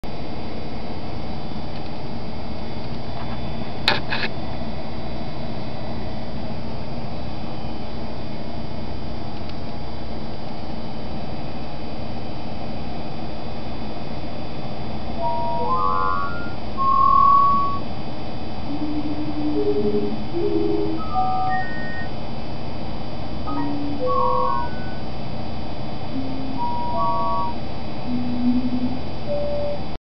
Sketcha-mo-phone, the circuit board, and arduino. Plus a little demo.
I show the circuit board used in my arduino sketcha-mo-phone musical instrument.